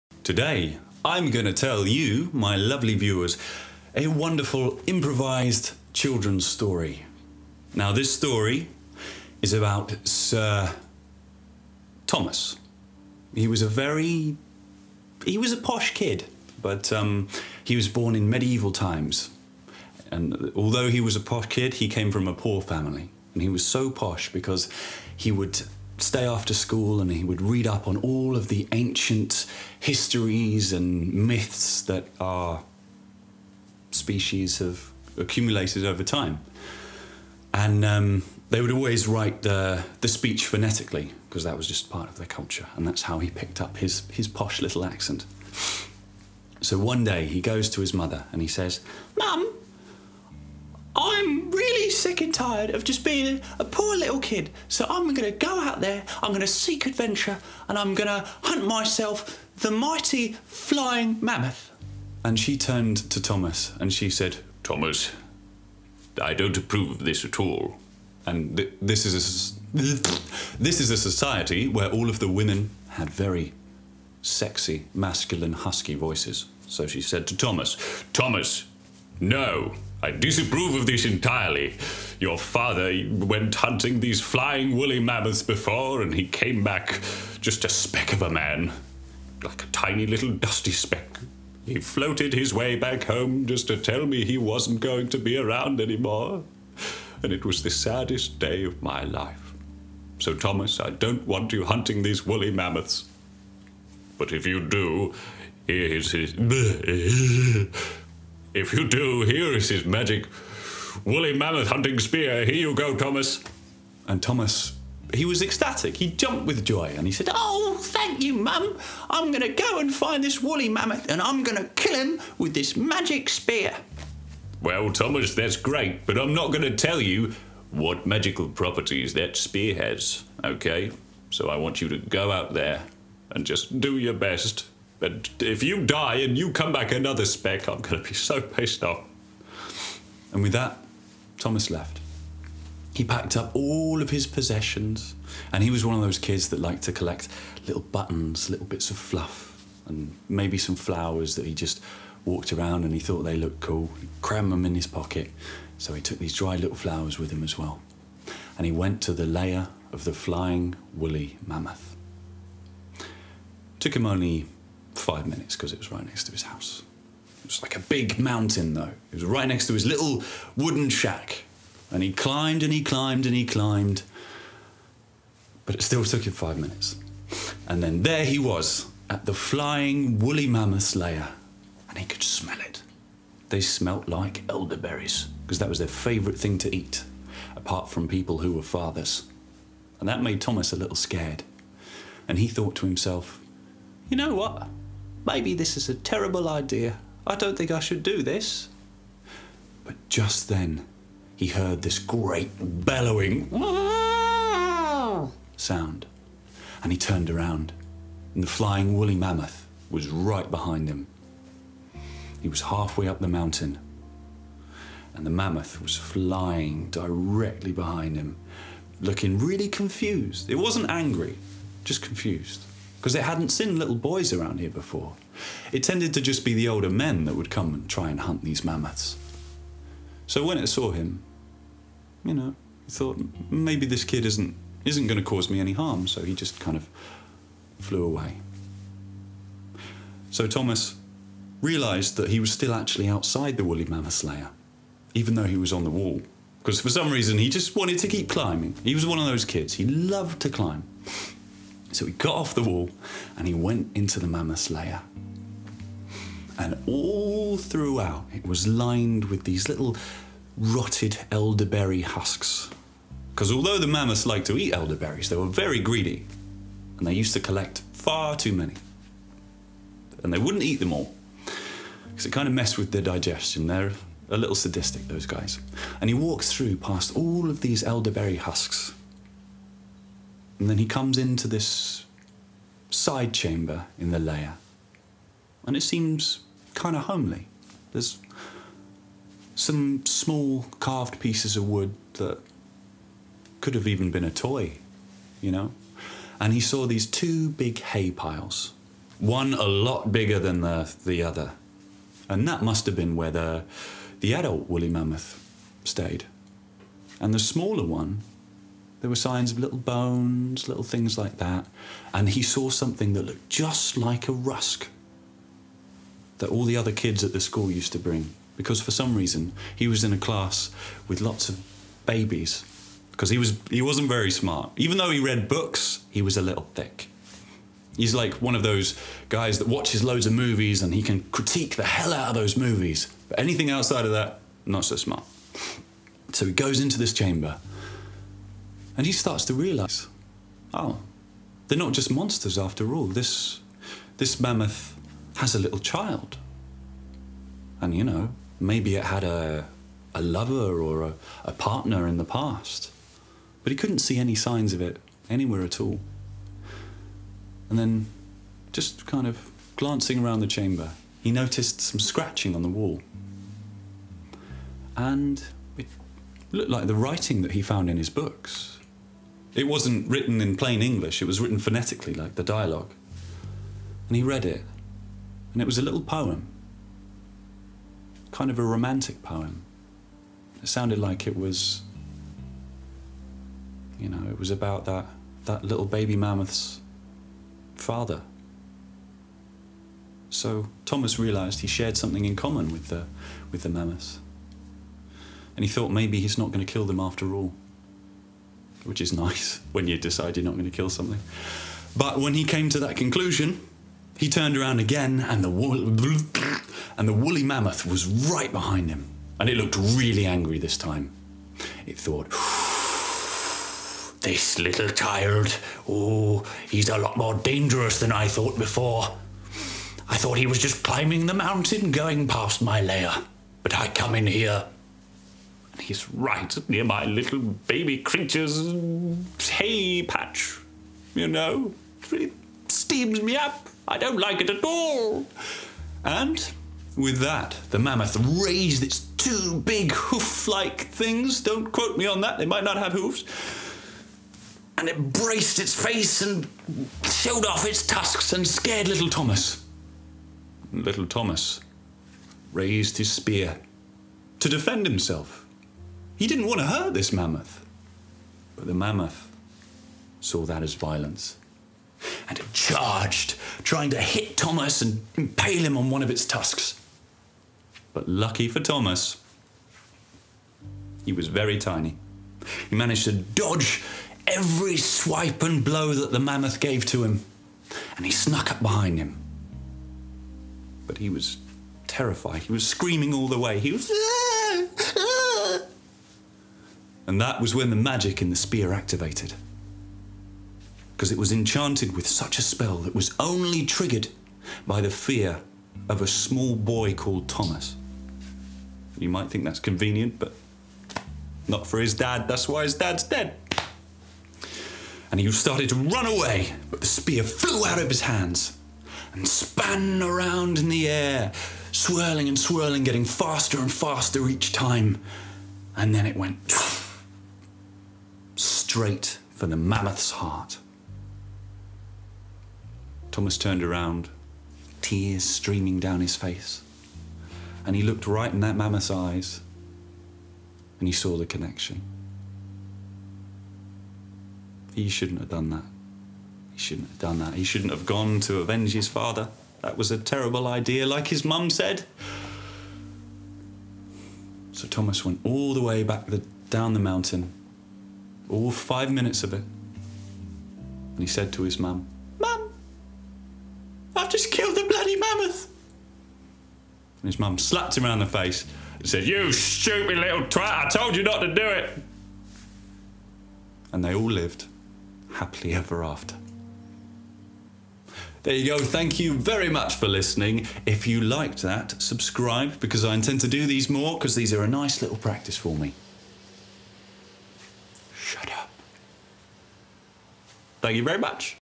Story Tellin' | Thomas & the Flying Mammoth
In the first episode of Story Tellin' I recall a tale from times long past.

(Had an issue with Recording & Export Settings, Potatovision™ was used only for the first two episodes)